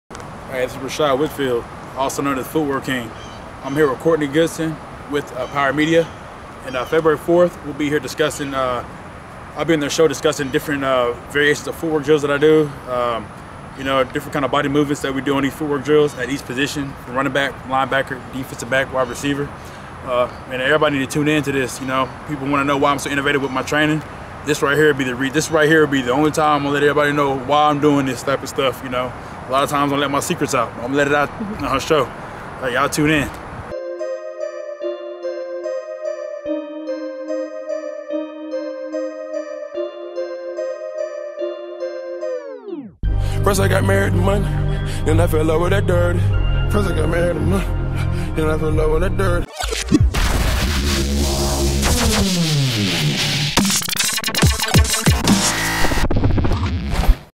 Rischad Whitfield "FootWork King" To Appear On Power Media Show
Well known trainer, Rischad Whitfield "FootWork King", will have a guest appearance on Power Media February 4, 2016.